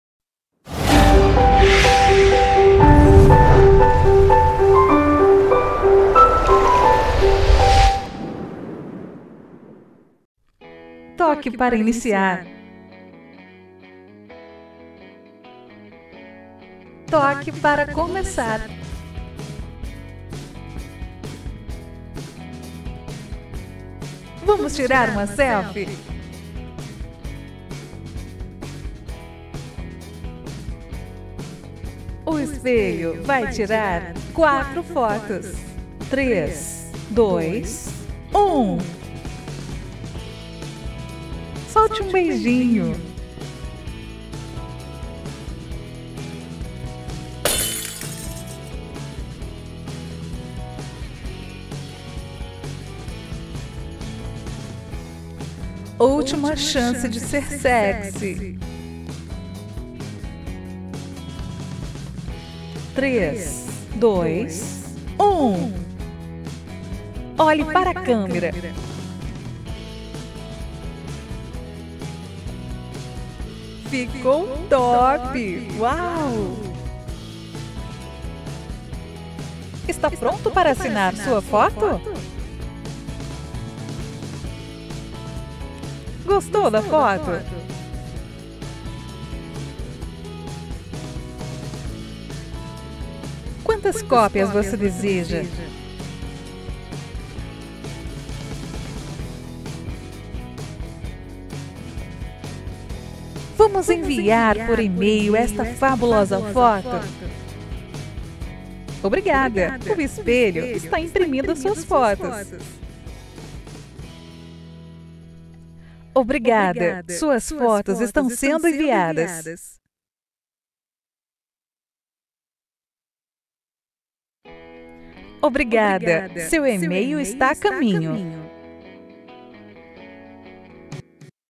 Animações Espelho Mágico com comando de voz - Programa Software Selfie Mirror Booth
Estas são apenas algumas das animações que fazem parte de nosso programa para Espelho Mágico.